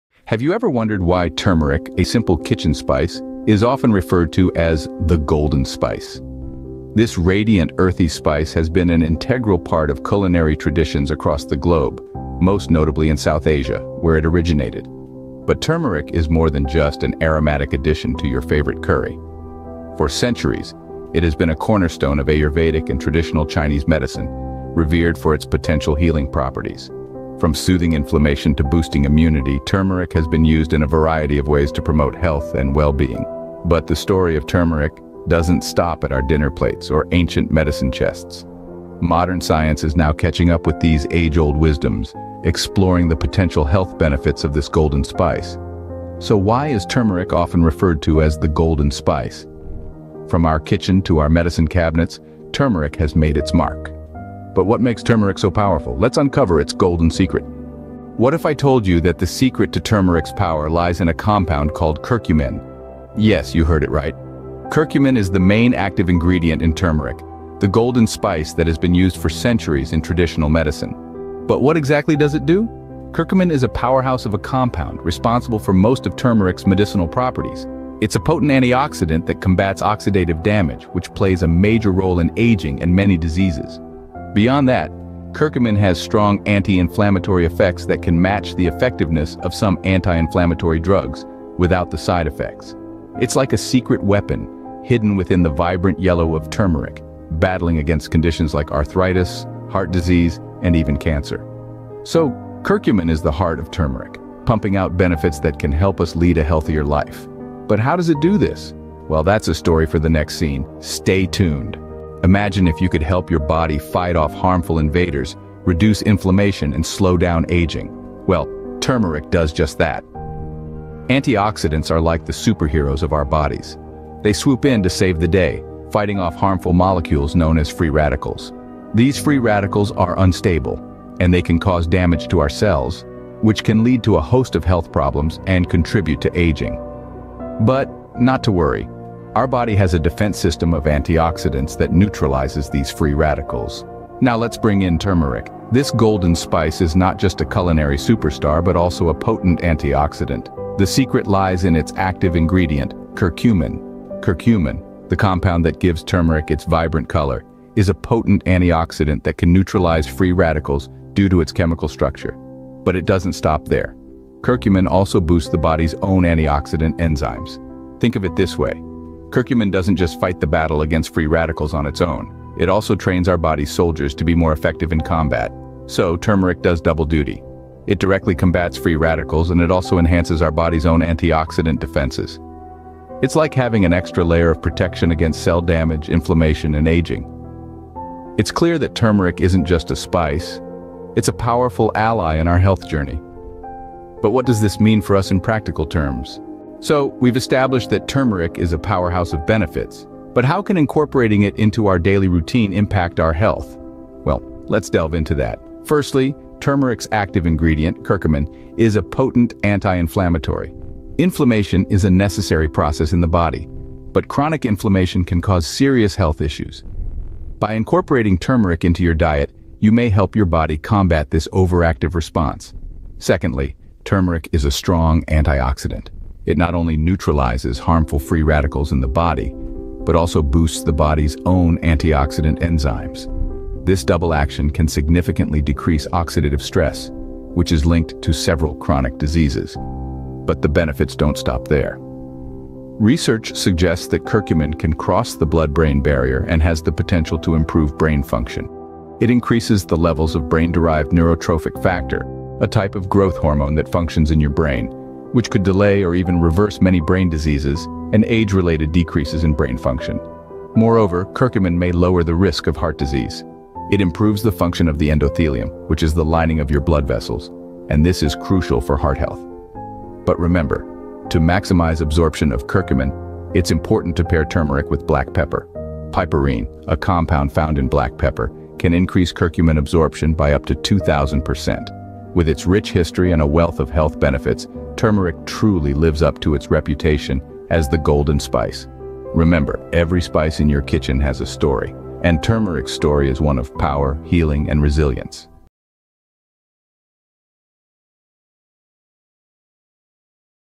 Turmeric: The Golden Spice of Personalized Medicine
In this video, we delve into the incredible health benefits of turmeric, its powerful antioxidant properties, and its fascinating role in personalized medicine. Turmeric has been used for centuries in traditional medicine and recent research highlights its potential to support overall well-being. Join us as we uncover the science behind turmeric's therapeutic properties and how it can be personalized to meet individual health needs. Don't miss out on this enlightening exploration!

OUTLINE:
00:00:00 A Golden Secret"
00:00:57 "Turmeric and its Active Ingredient"
00:01:51 "The Antioxidant Properties of Turmeric"
00:03:16 "The Practical Benefits of Turmeric for Health"